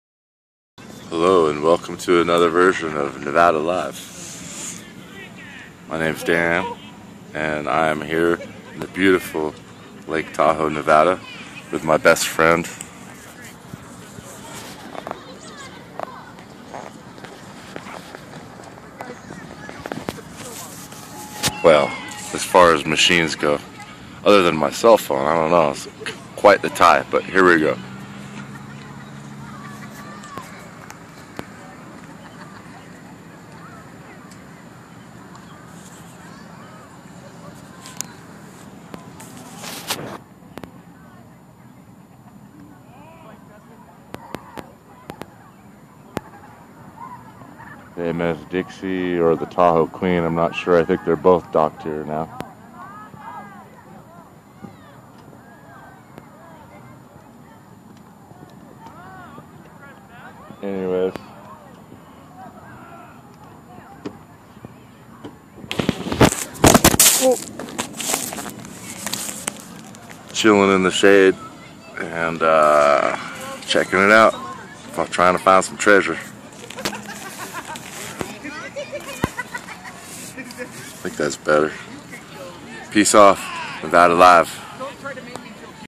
Nevada "Live" Dan C Zephyr Cove, Nevada
Detectors right on! Few quarters pennies, bottle caps, and Bobby Pins. Temps perfect!!